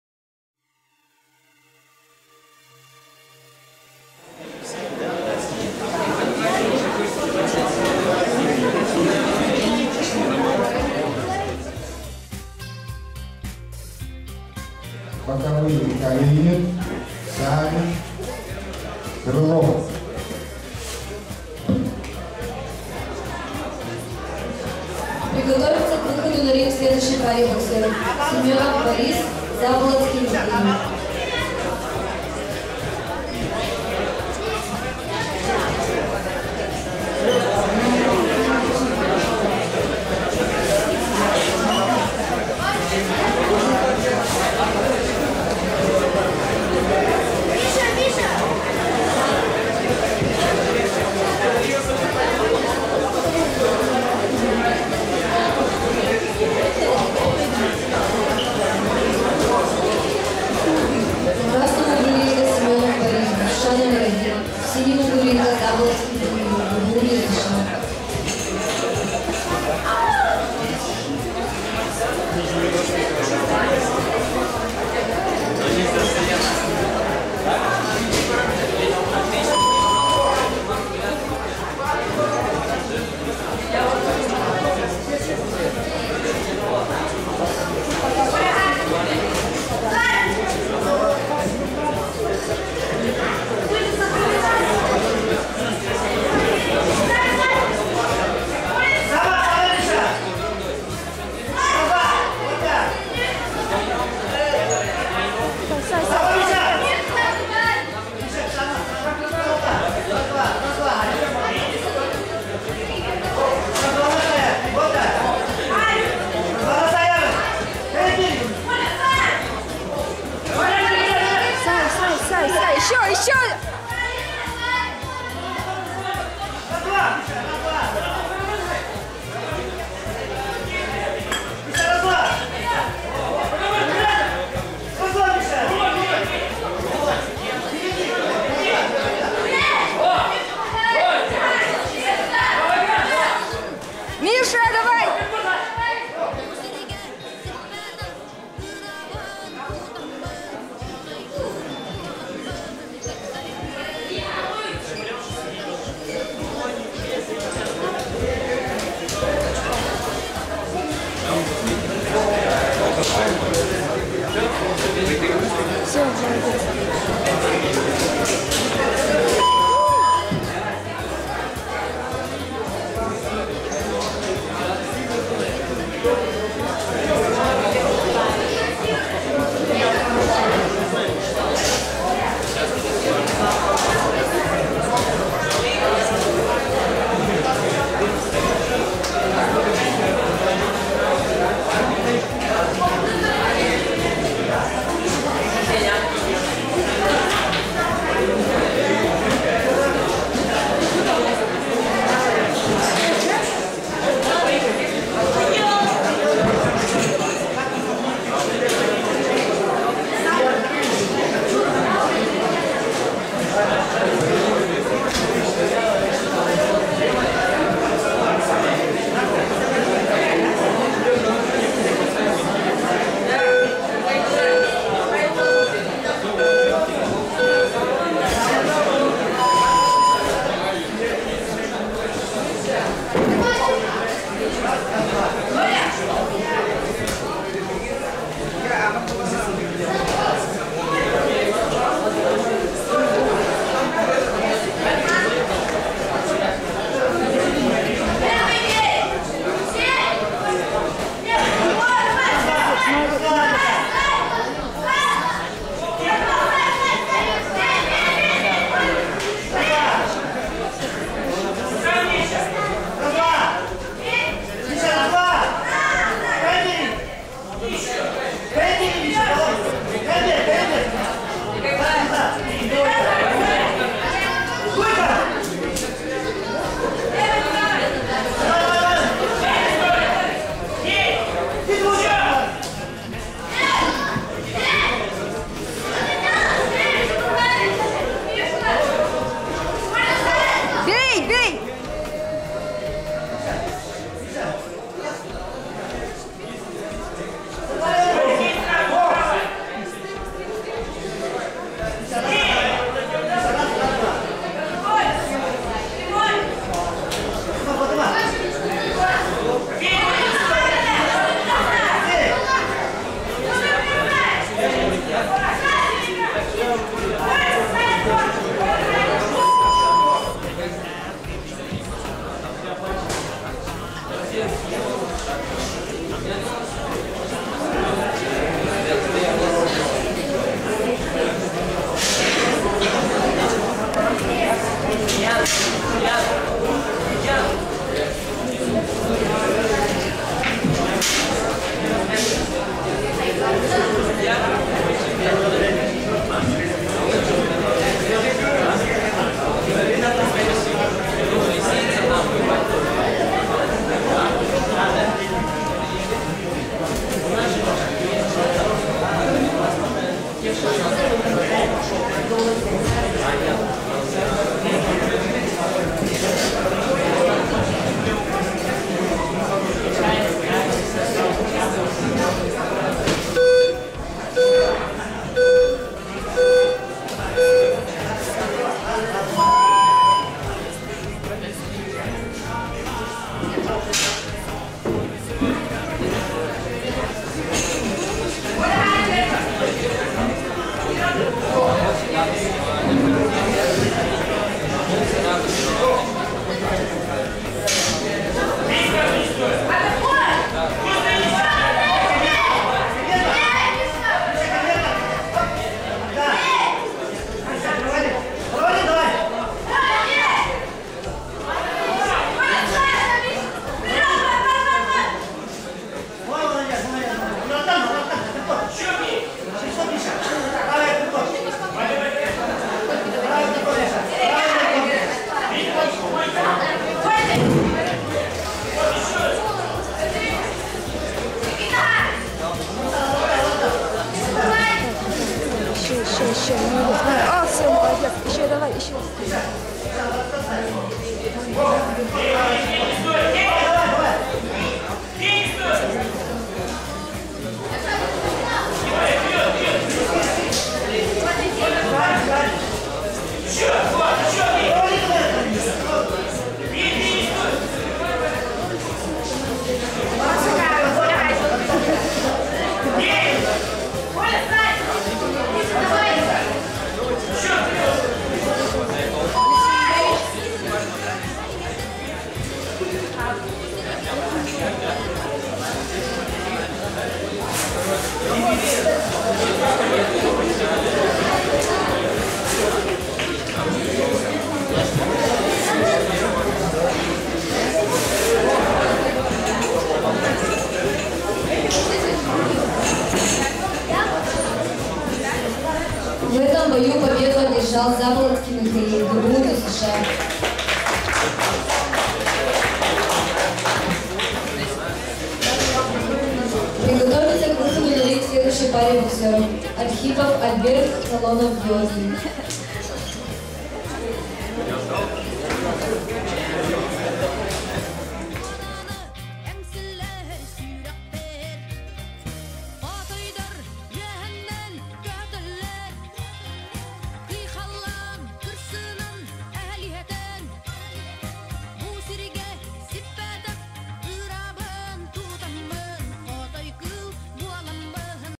БОКС
Финал, первенство по боксу г. Якутска 2014 г. памяти МС СССР И. Кураева. (первые бои)